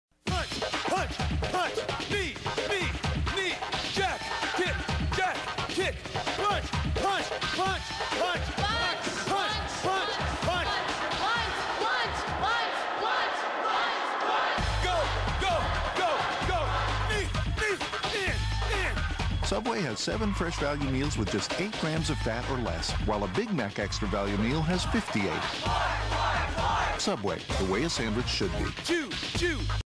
2000 Subway TV Ad w/Billy Blanks
Punch and kick your way to healthier eating. sounds good...

I am having a creative outburst and uploading a lot of videos recently...